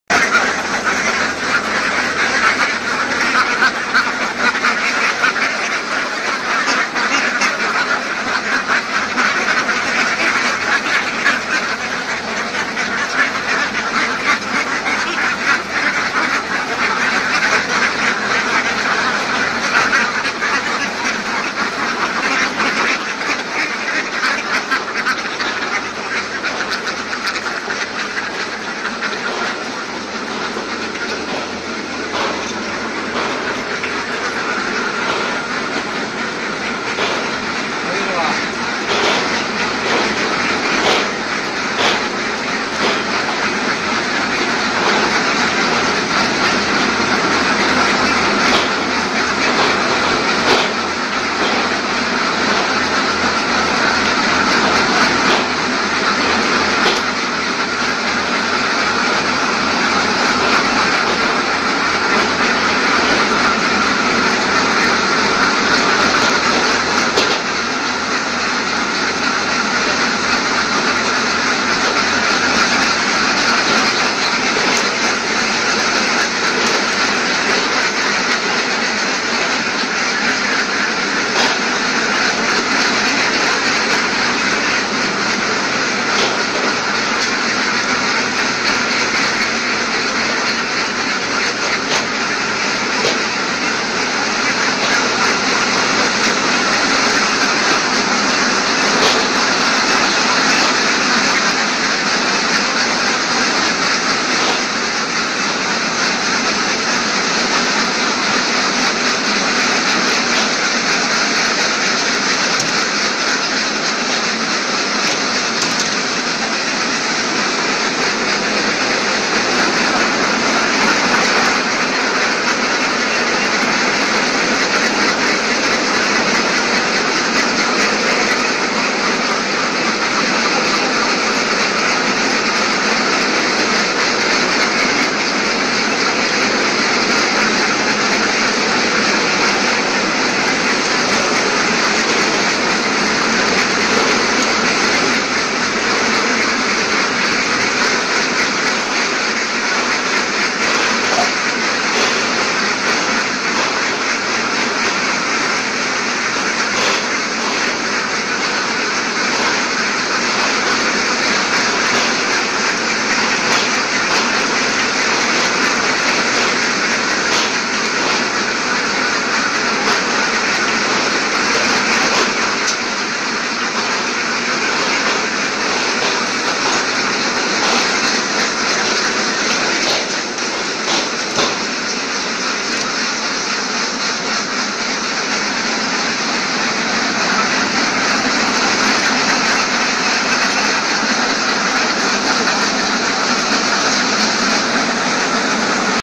Automatic duck cages system
Duck farming with automatic drinkers, feeders and automatic egg collector.  There should be further setup for the automatic egg collector, but the customer think this is ok.
The similar system also applies to layer chicken.
With this system, you only need 1-2 people to manage 10000 birds